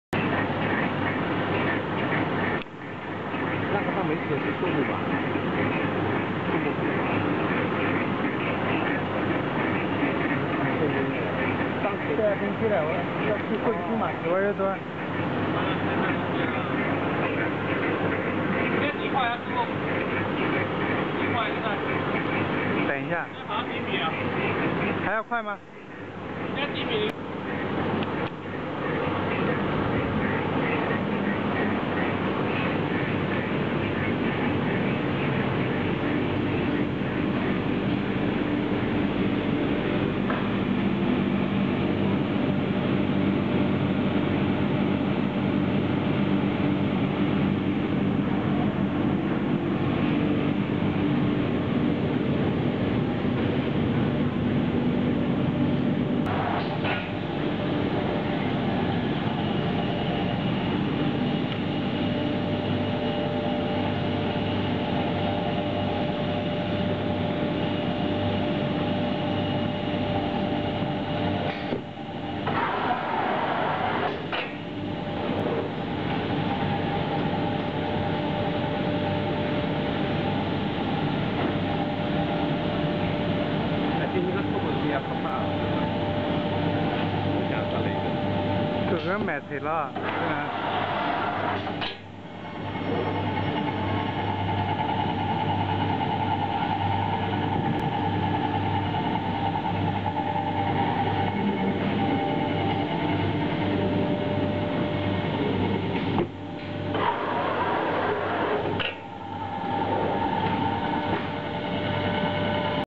Vertical corrugating lamination line
This lamination line, can be used as honeycomb paperboard laminating as well.